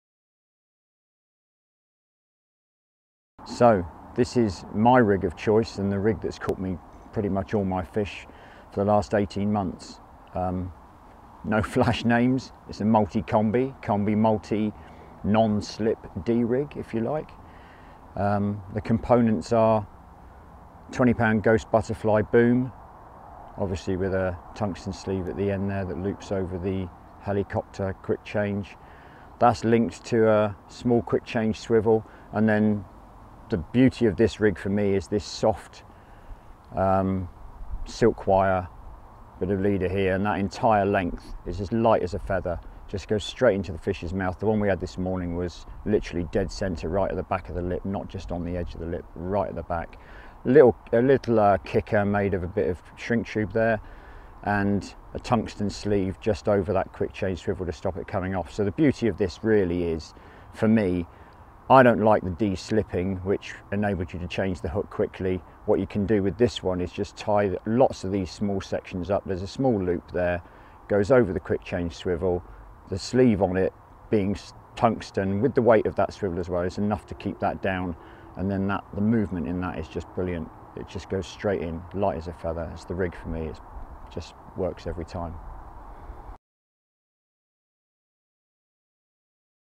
STEVE PINNS RIG
Steve Pinn takes us through his variation of the Slip - D rig and why he uses it